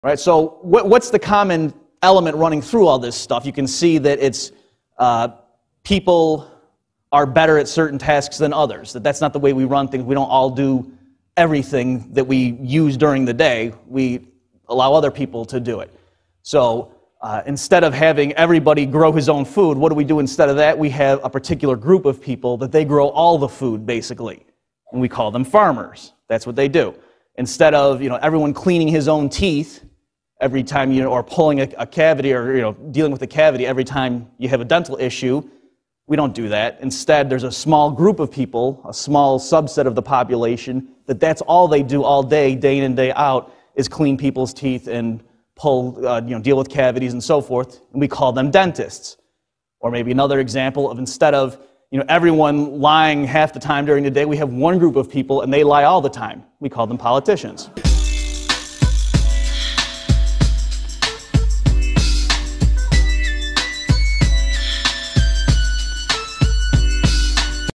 Robert Murphy explains role of politicians in division of labor
So to speak!

For full lecture, search: Robert P. Murphy - The Division of Labor